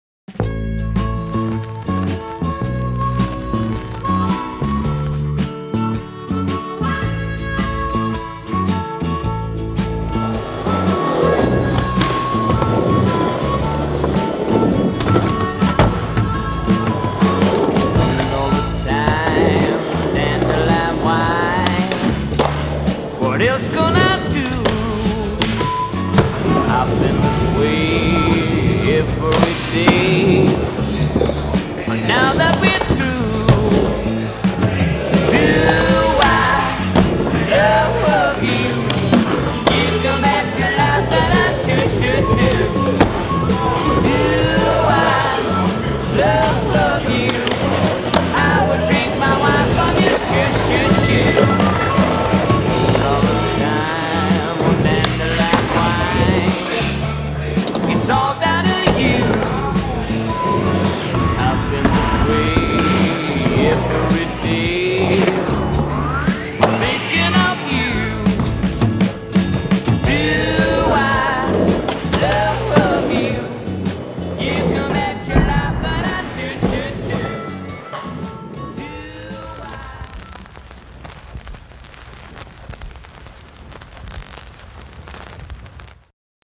Afternoon In The Park: Madars Apse - TransWorld SKATEboarding
Madars Apse came through our park to get a session in the Anniversary Edition DC Lynx. His homies Wes Kremer and Sam Hubble got in on the action and opened up some NBDs in the park.